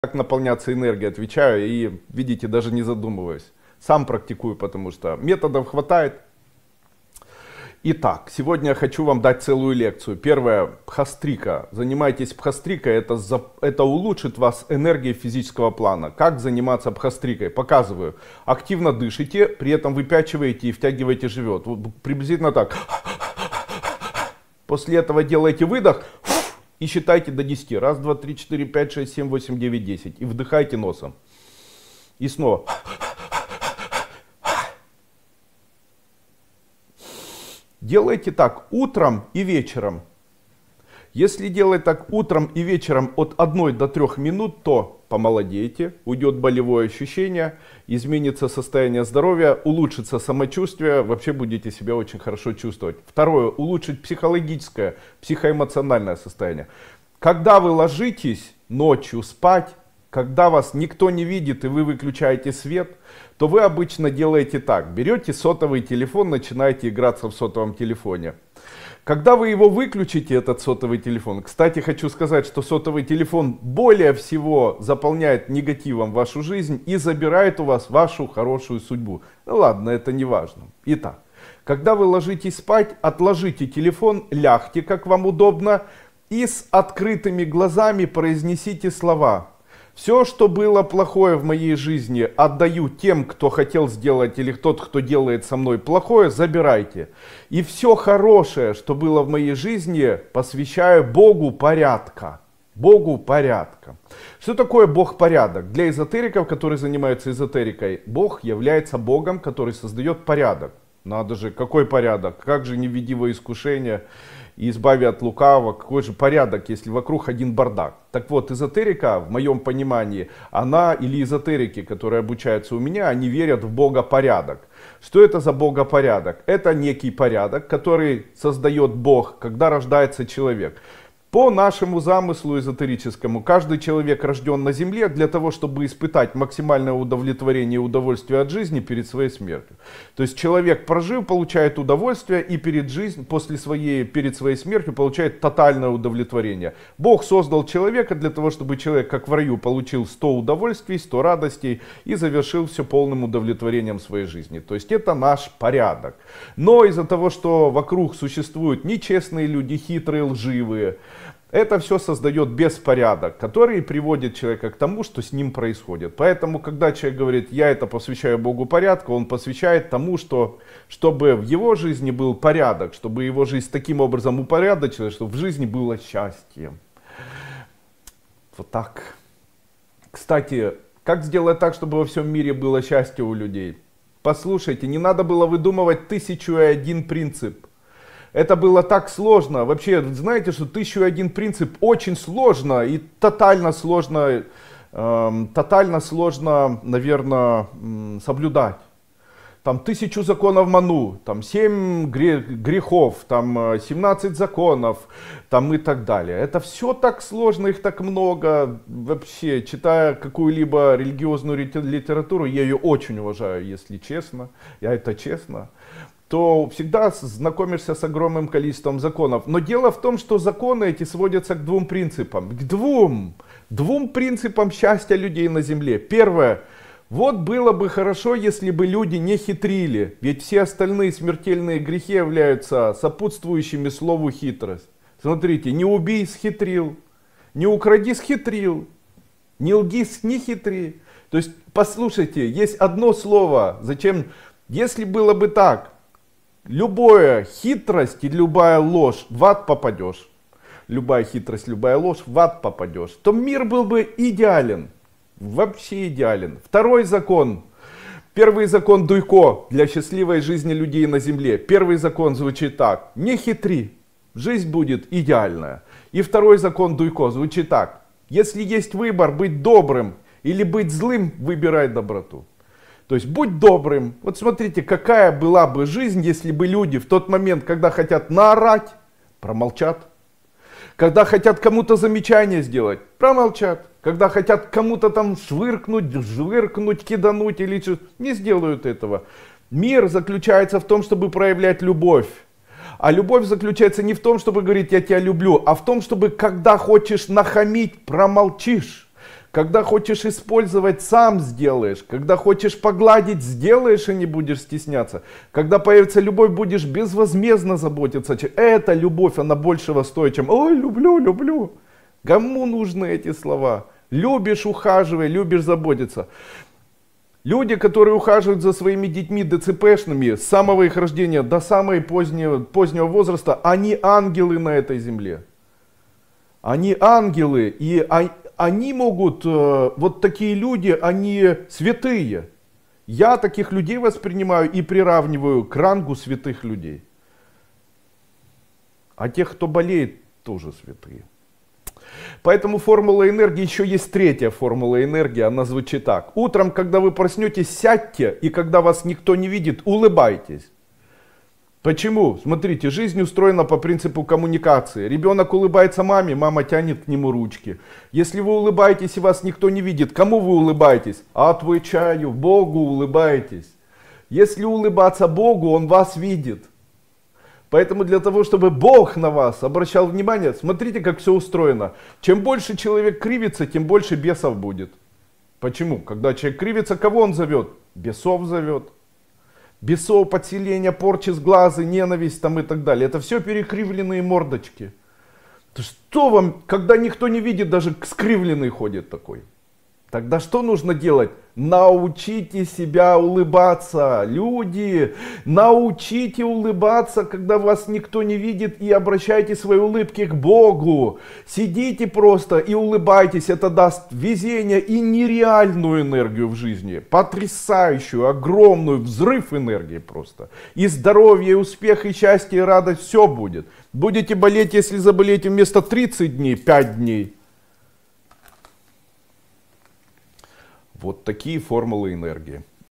Три Метода Наполнения Энергией: Путь к Тотальному Удовлетворению Жизнью
В этом видео рассматриваем три мощных метода наполнения энергией, которые приведут вас к тотальному удовлетворению жизнью. Подробно объясняем, как использовать метод дыхания бхастрика для моментального заряда энергии. Исследуем практику посвящения хорошему перед сном, вручение всех благ Богу Порядка. А также, рассматриваем третью формулу энергии - утреннюю улыбку, как способ получения нереальной энергии для улучшения психоэмоционального состояния.